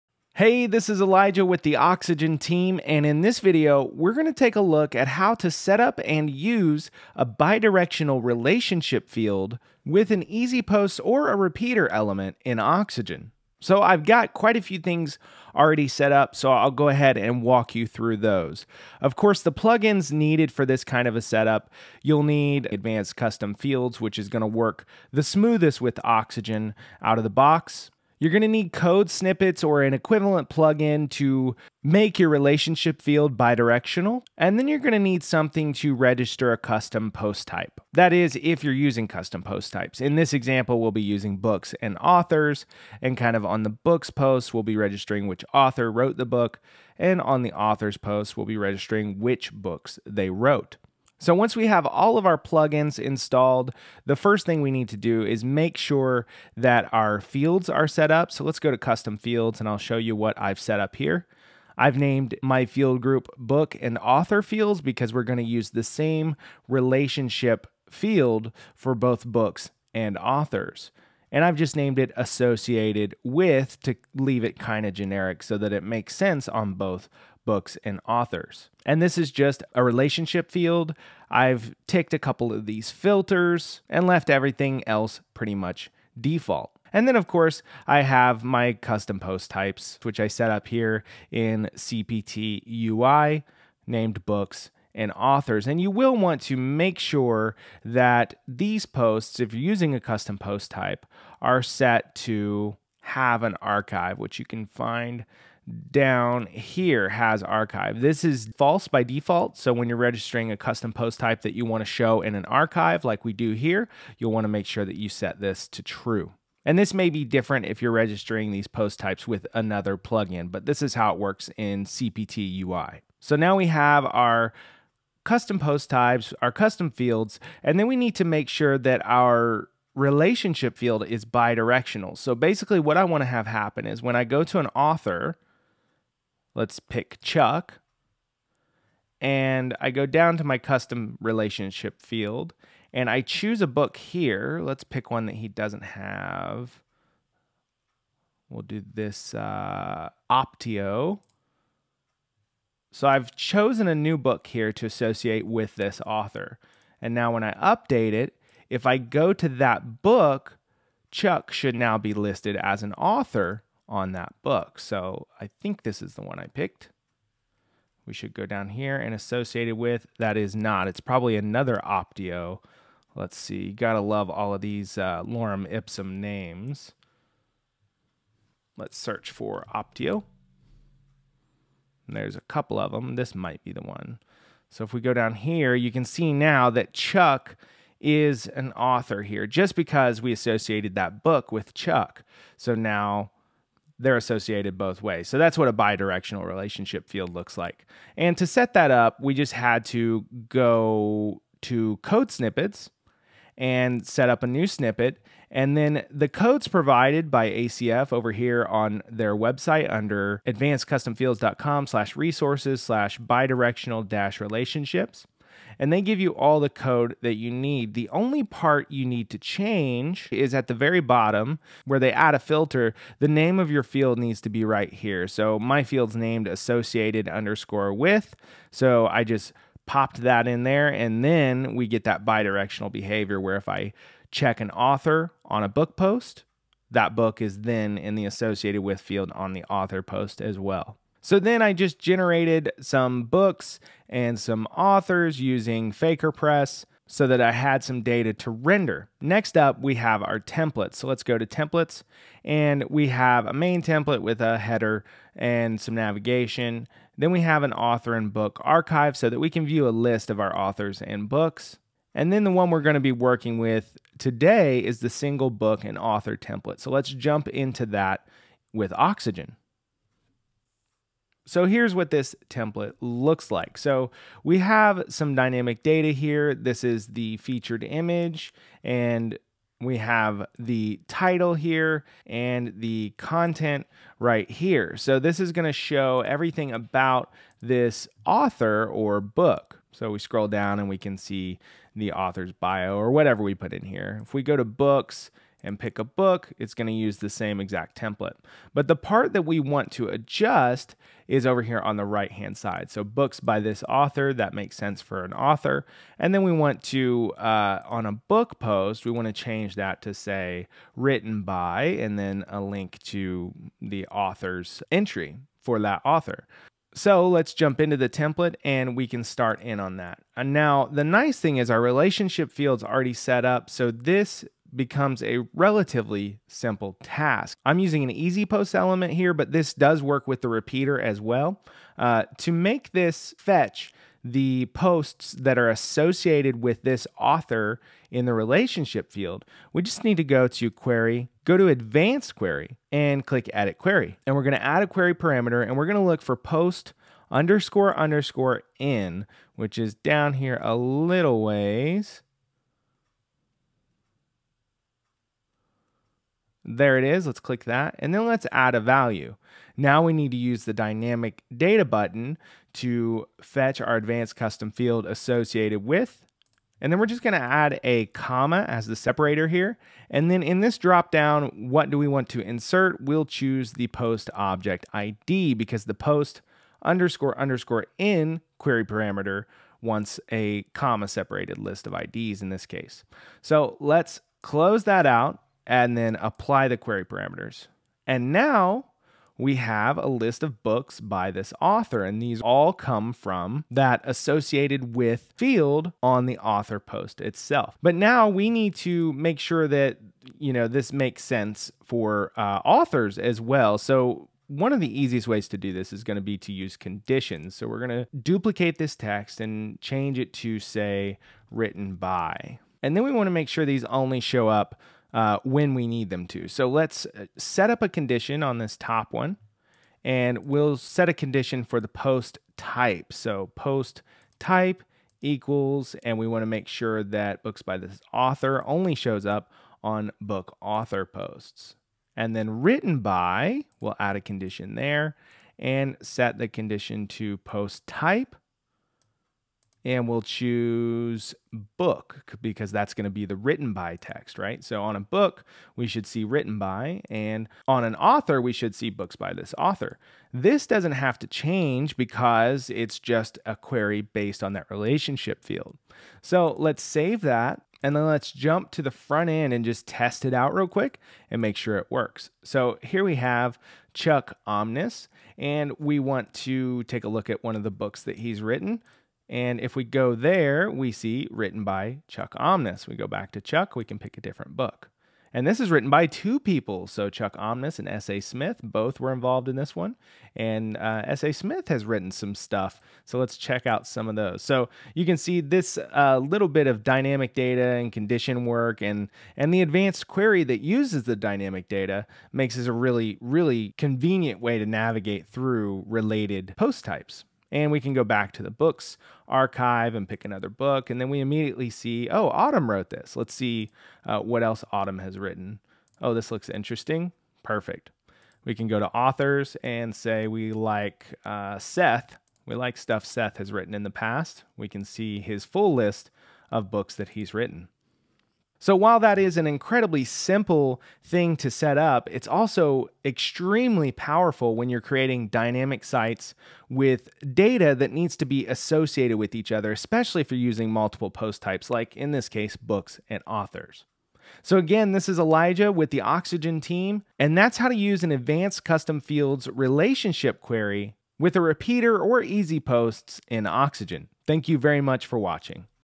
Using ACF Bi-directional Relationship Field In WordPress with Oxygen
In this video see how to use Oxygen's Advanced Query feature in an Easy Posts or Repeater element to easily show posts from an ACF bi-directional relationship field.